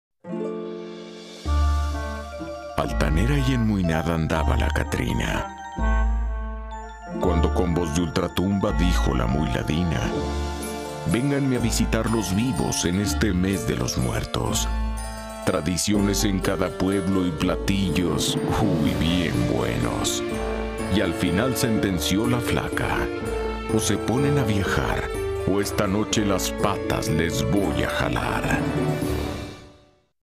Dia de Muertos 2008 turismo de mexico
Spot de promoción turística de mexico emitido en televisión durante octubre del año 2008 haciendo promoción a la festividad del Día de muertos como punto de visita para el turismo.